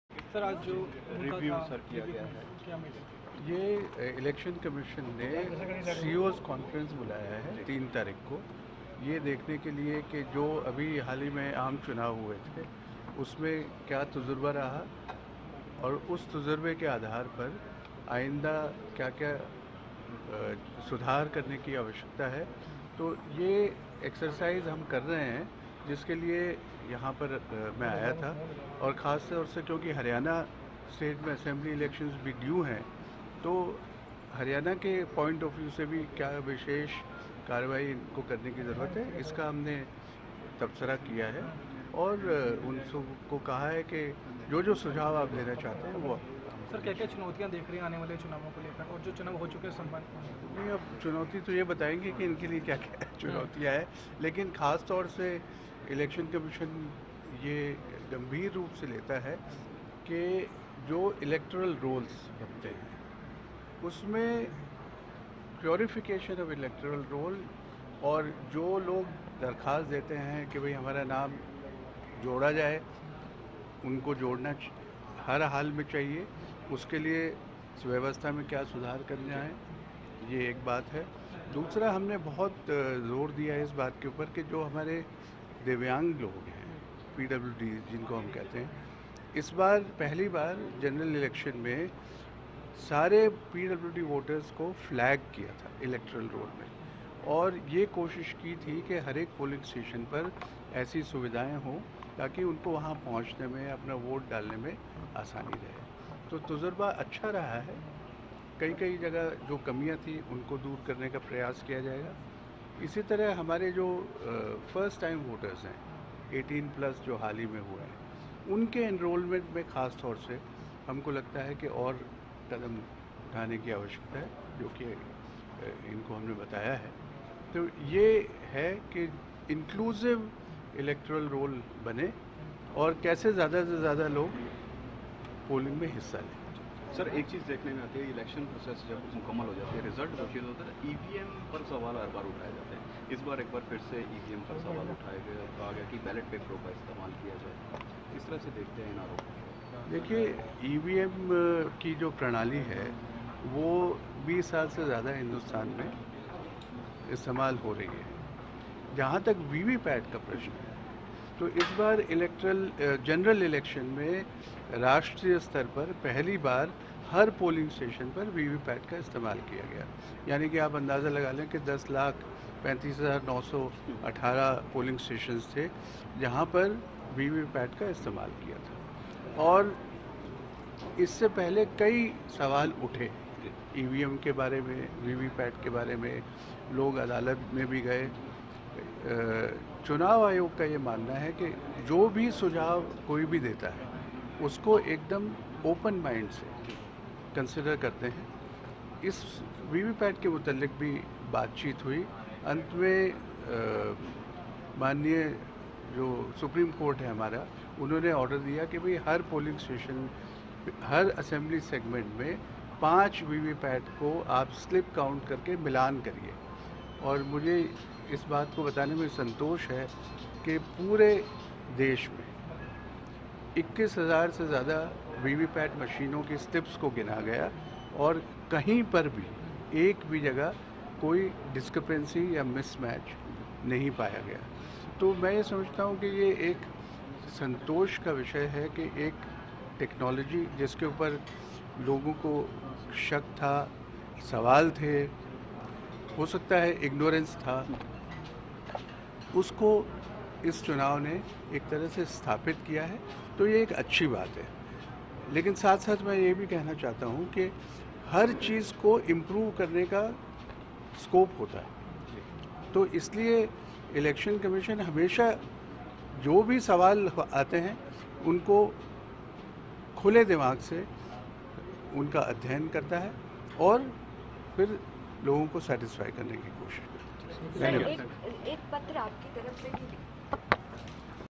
CHD ELECTION COMMISSION ਕਿਹਾ ਚੋਣਾਂ ਨੂੰ ਲੈ ਕੇ ਕਿਸੇ ਪ੍ਰਕਾਰ ਦੀ ਨਹੀਂ ਹੋਈ ਹੇਰ ਫੇਰ
Punjab Shiromani Akali Dal
Captain Amarinder Singh
hs phoolka
sukhpal singh khaira